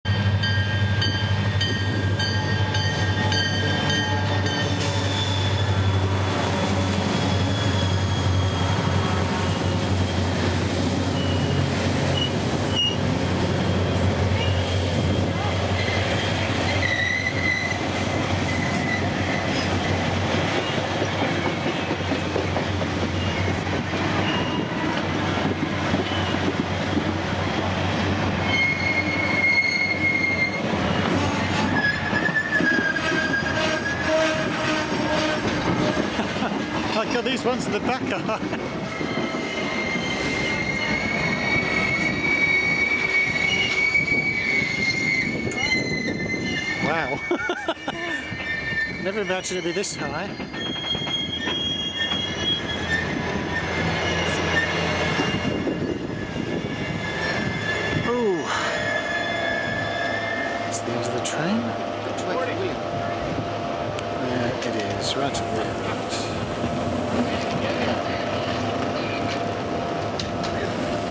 Arrival of Rocky Mountaineer train at Banff station, September 2009
Arrival of Rocky Mountaineer train at platform. One minute, 12 secs length.